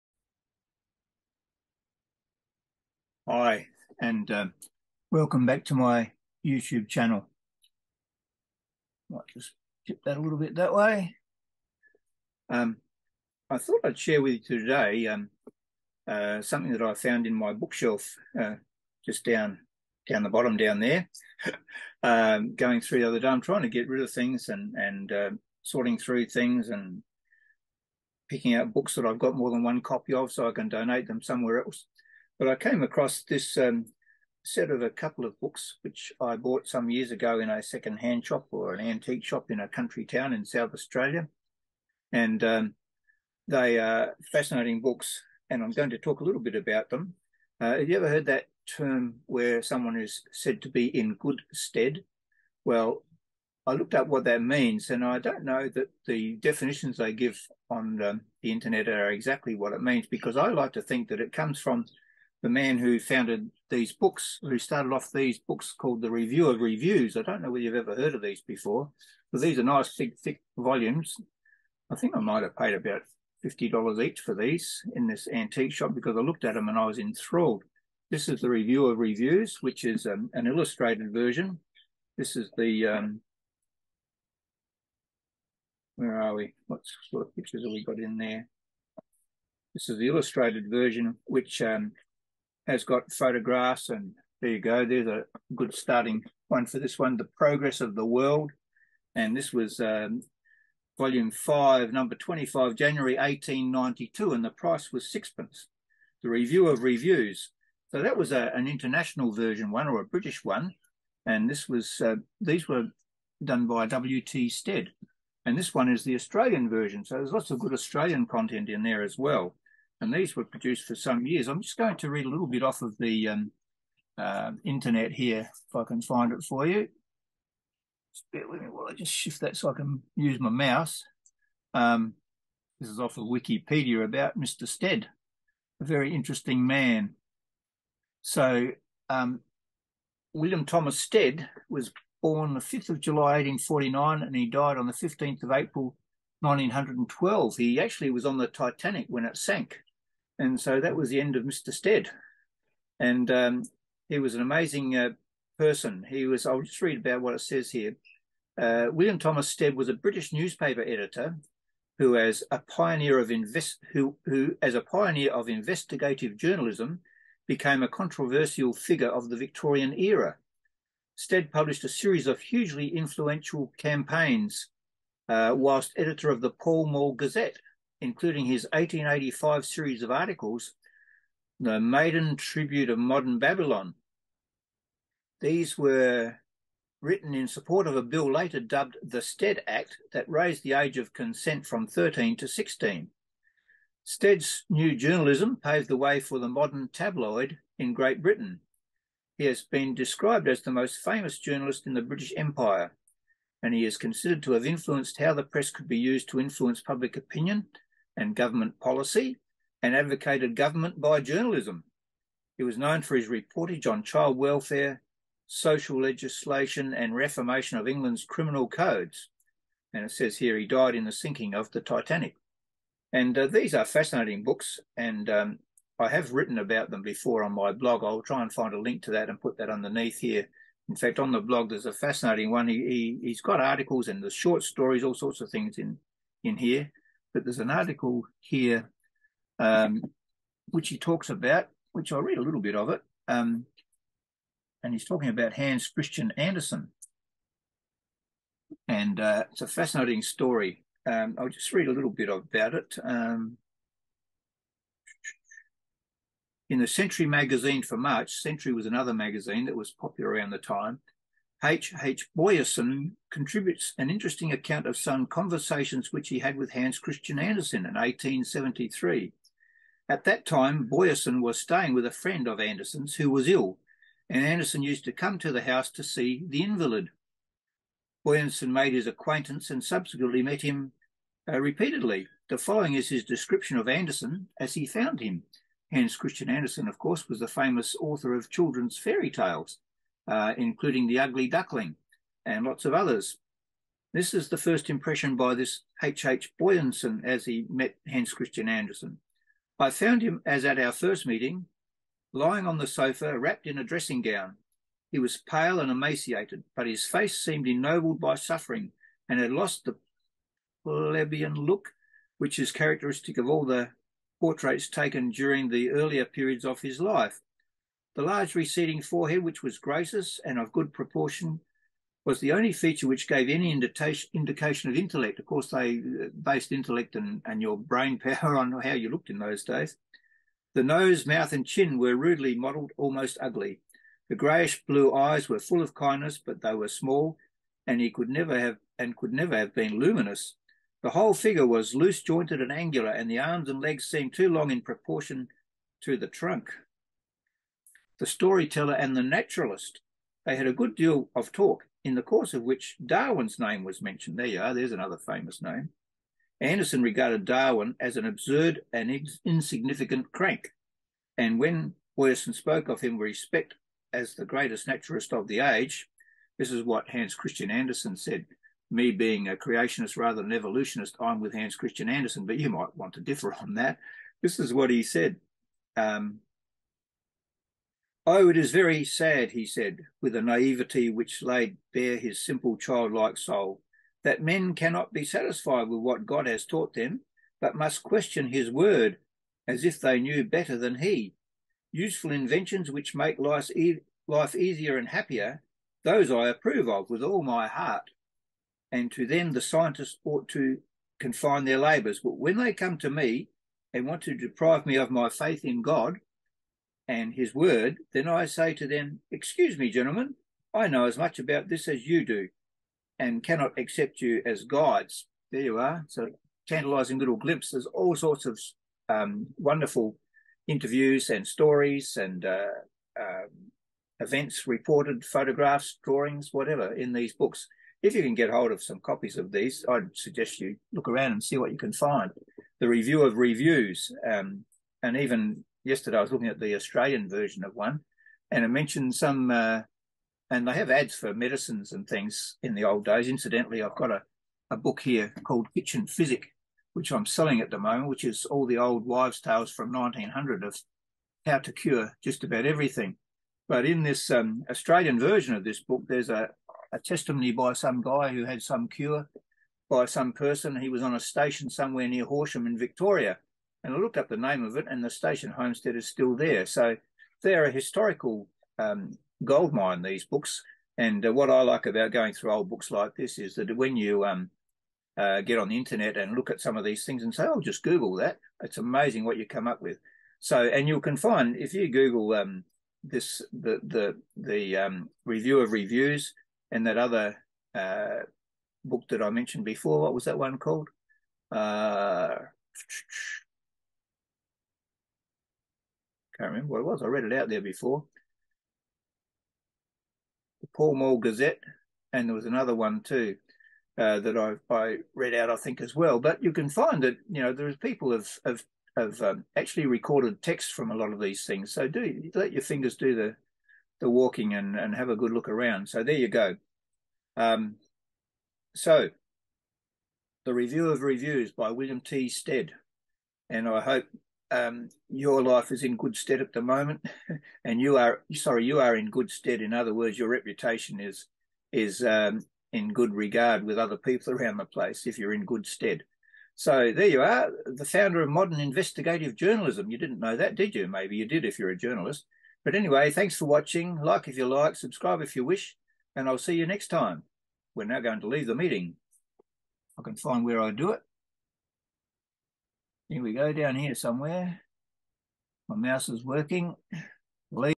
"The Review of Reviews" by W T STEAD
This video is about "The Review of Reviews"  by W T STEAD
They say he was the founder of modern investigative journalism.
I have written about this on my blog in the past.
Let you own investigations proceed.